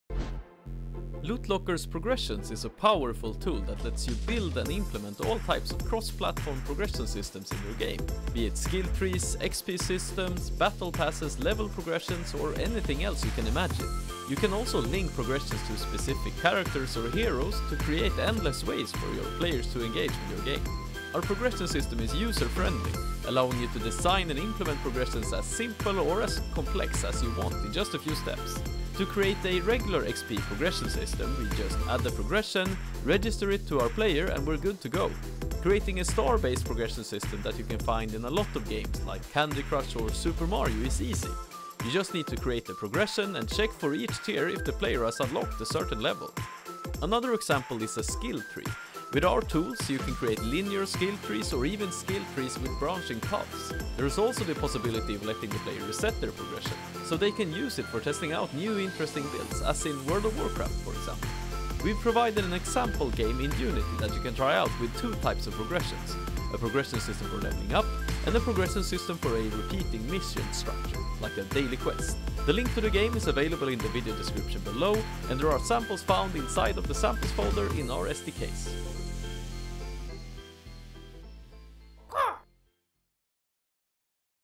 Progressions - Feature Trailer
Use progressions to create skill trees, level up systems, character upgrades, quest lines and more. Fits all types of games such as RPGs, strategy games, adventure games, but can also be useful for other games such as sports or simulation games. Can be used together with a reward system for your game to handle experience points, skill points, mastery levels, quest rewards, boss drops, random loot, difficulty scaling, and more for your game's progression system to increase player engagement, satisfaction, and retention. Build a sense of achievement and reward players for their effort, making your game more immersive and enjoyable.